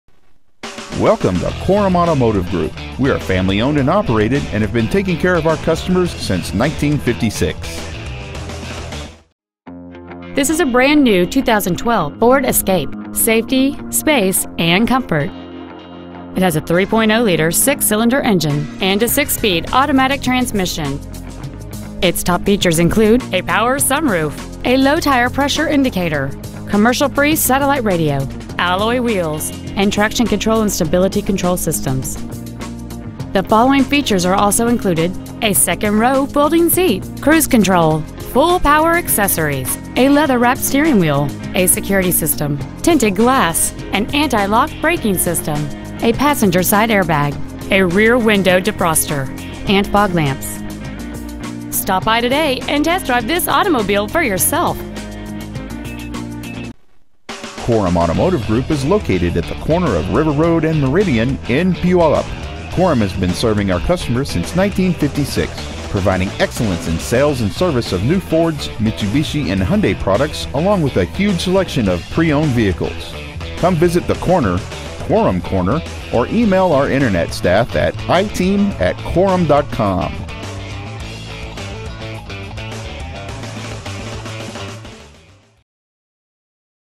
2012 Ford Escape Puyallup WA 98371
Year : 2012
 Make : Ford
 Model : Escape
 Engine : 3.0L V6 DOHC 24V
 Trans . : 6 Spd Automatic
 Exterior : Sterling Gray Metallic
 Miles : 25
 Interior : Charcoal

 2012 Ford Escape Puyallup WA